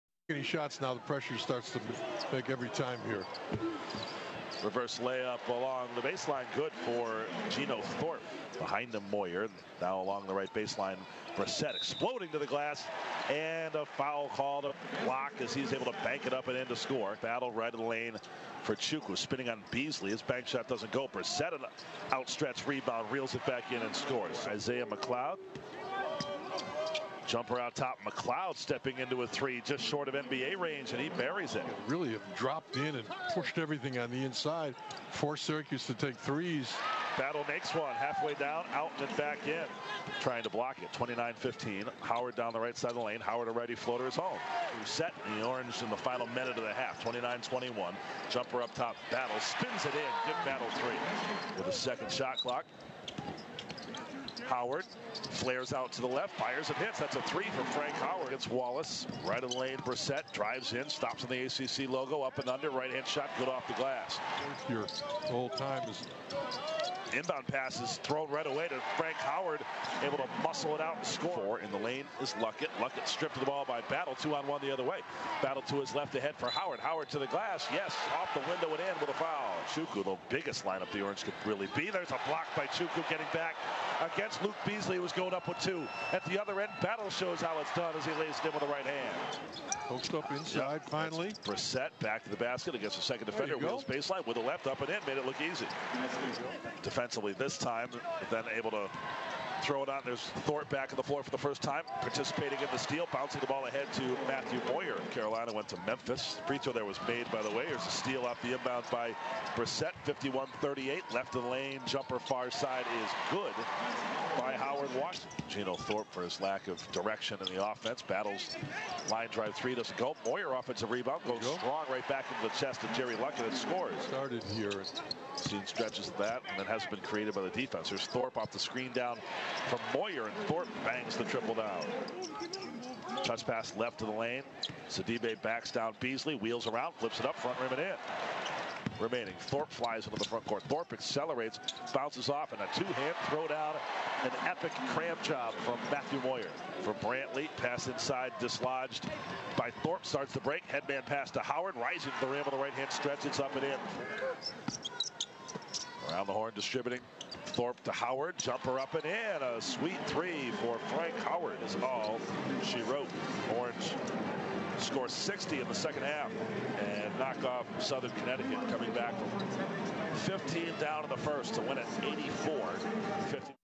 Highlights | Syracuse vs. SCSU
Highlights from the Syracuse men's basketball team's 84-59 win vs. Central Connecticut State in the Carrier Dome.